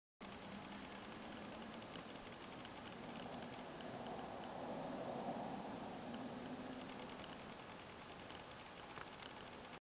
Mule deer eating lichen from branches of a felled Jack pine